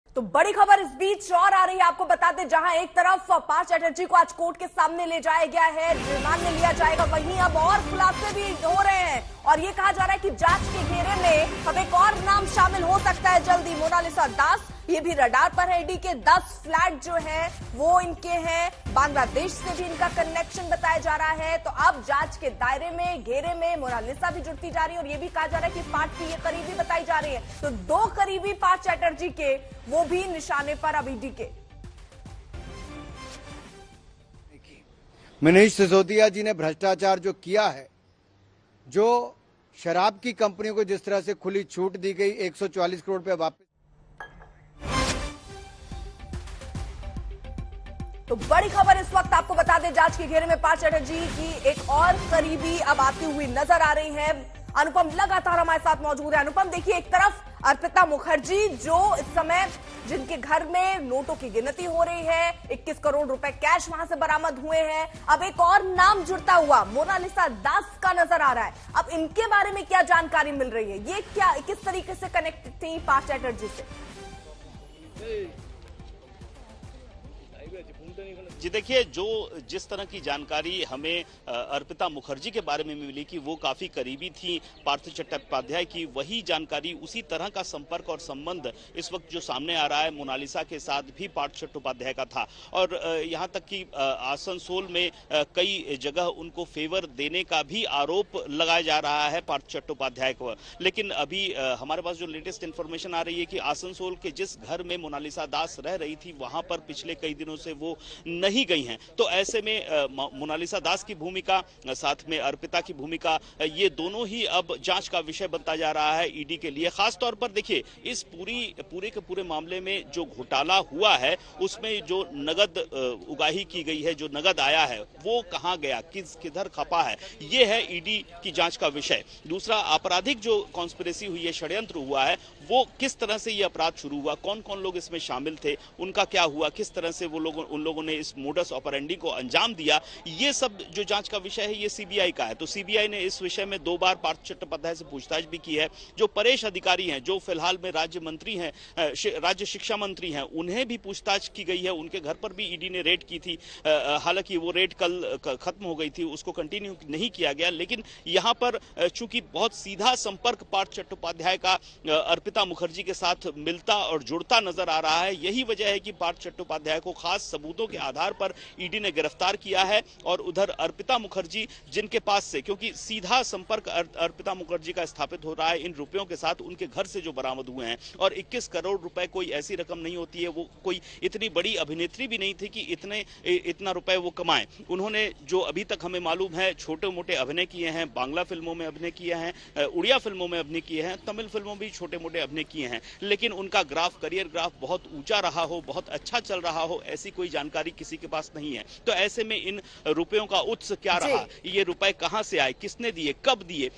ED: जांच के घेरे में पार्थ चटर्जी की एक और करीबी | Partha Chatterjee | Arpita Mukherjee | Monalisa
पश्चिम बंगाल में टीचर भर्ती घोटाला में गिरफ्तार पूर्व शिक्षा मंत्री पार्थ चटर्जी की एक और करीबी प्रवर्तन निदेशालय (ED) की रडार पर है.